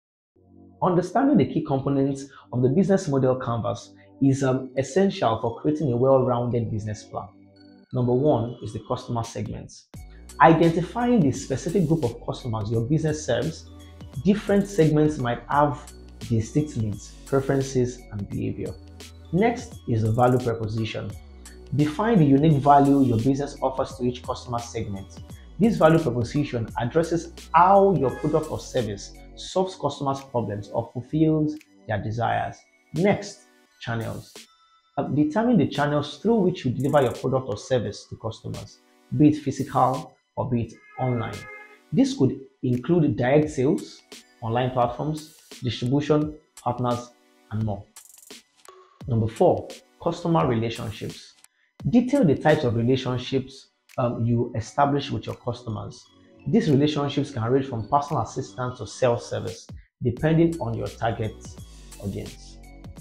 Writing a Business Plan (Lesson 11): Key Components of the Business Model
Delve into the essential components that form the foundation of a successful business model! 🚀 Understand the significance of elements like value proposition, customer segments, revenue streams, and key resources in effective business planning. 🌟 Explore the vital features that define a strong business model framework and its role in market analysis, innovation, and startup success. Start your journey to comprehending the key components of the business model today!